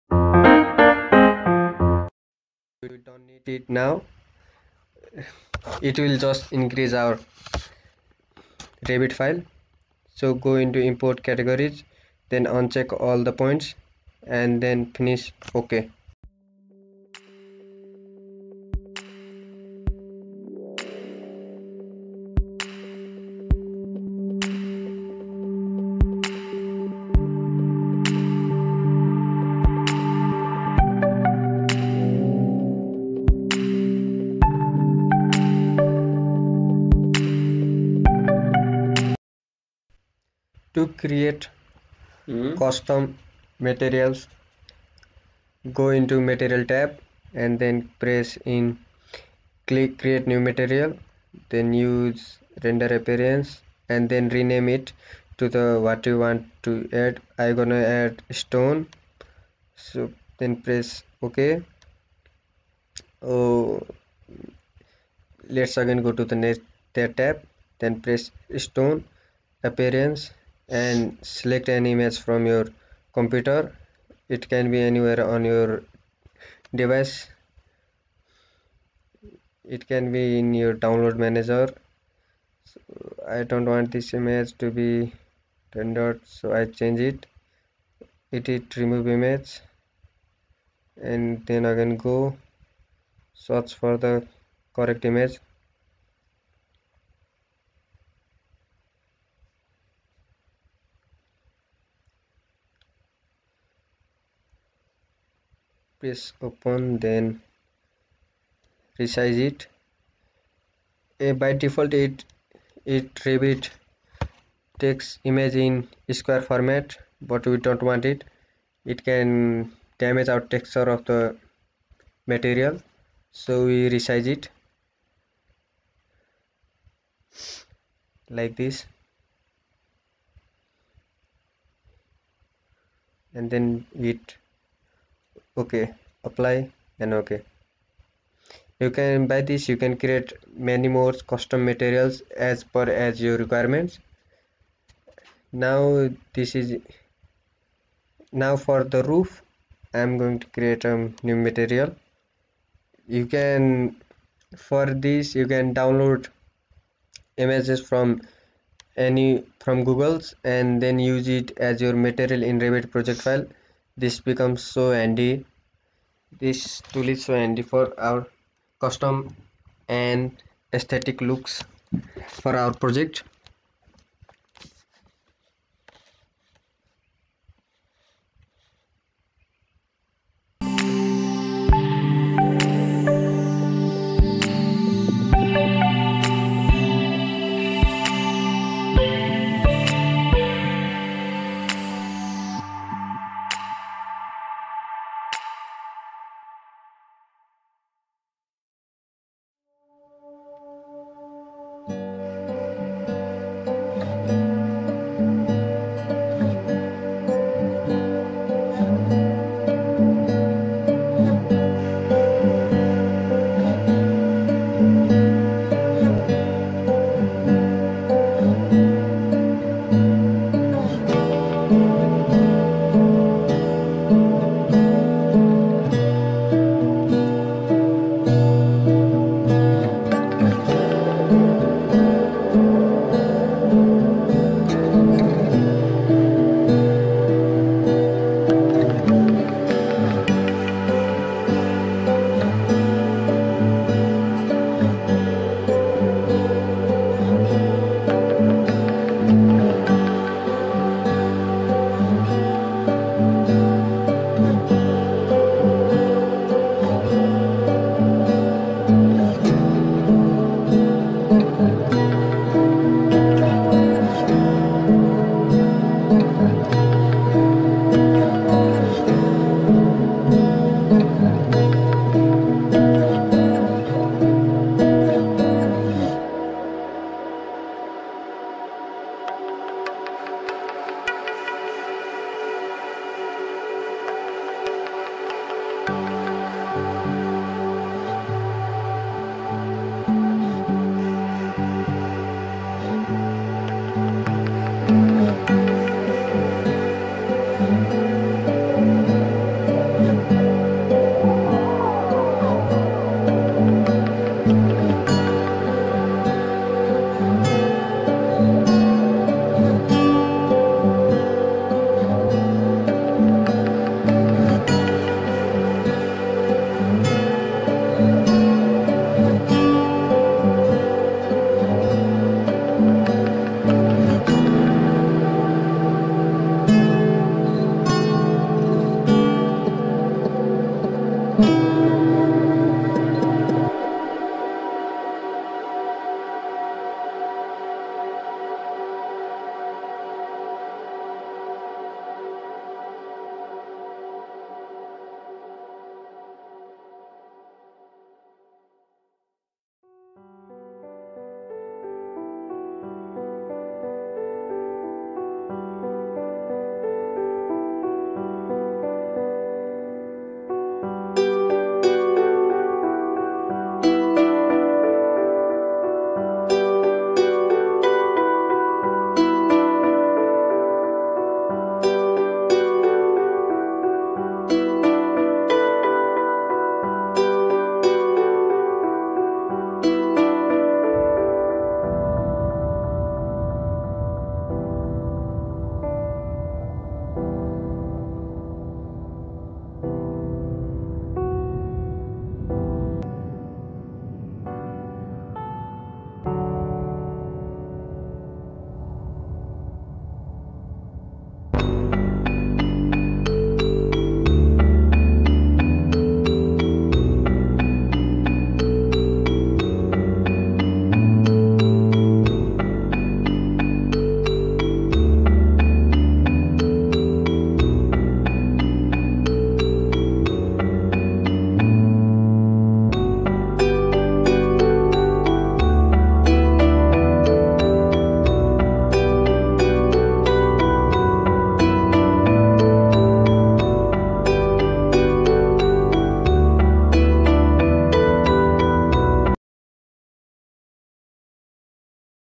drawing 3d model in revit with the help of cad floor plan part 3 ||006
hi guys,
in this video i have created a 3d model of a house based on the floor plan made in the autocad.when you use a dwg floor it becomes so easy to build your 3d model.hope you like it.